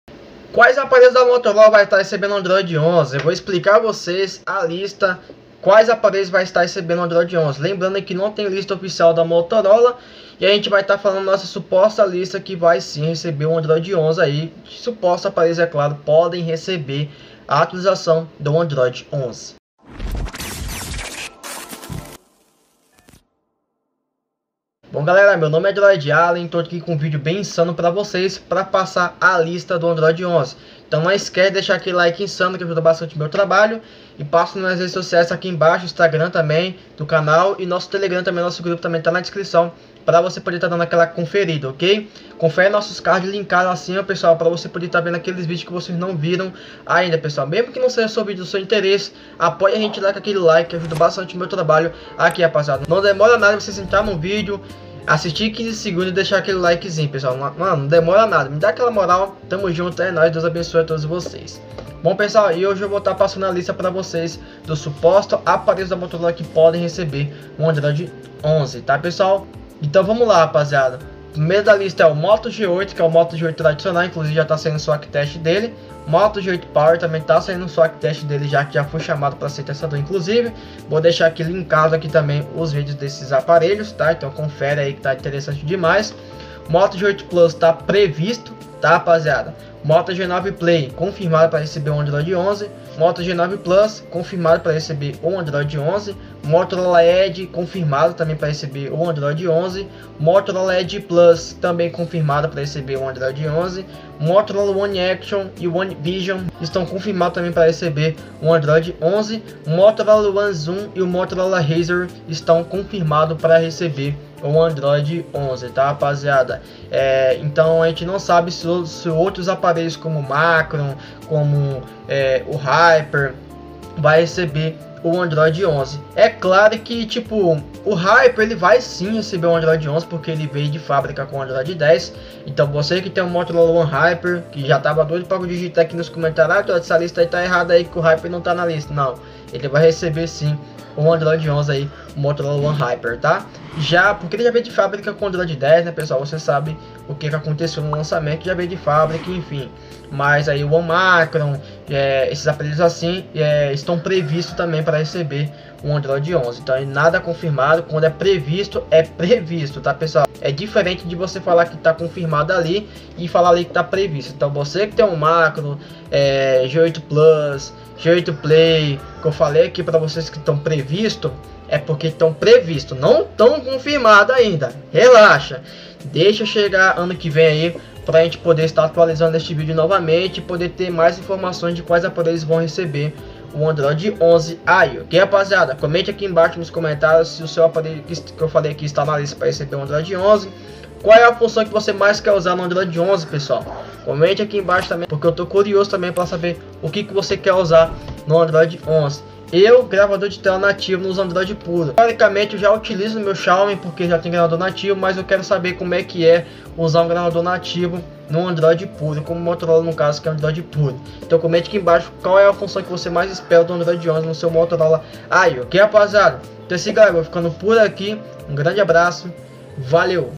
LISTA ATUALIZADA! QUAIS APARELHOS VÃO RECEBER O ANDROID 11 DA MOTOROLA? - Droid Allen
VÍDEOS SEGUNDA A SÁBADO 16H & 20H.

E-MAIL PARA PATROCÍNIO.

⚡ MEU NOME E DROID ALLEN ⚡

 MOTOROLA BRASIL NO TELEGRAM!